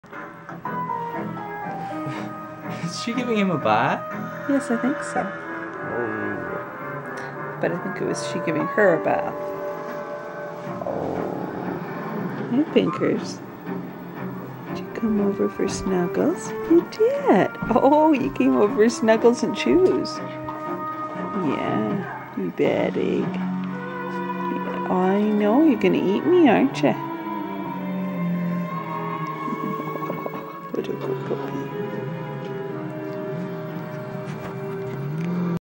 Red and black standard poodle puppies being oh so cute #12
Arreau's I'll Follow the Sun (Betty Jo) and Winnow's Happy Go Lucky at Arreau (Quincy)'s red standard poodle puppies at almost 3 weeks old.  For more really cute pics and informations please visit our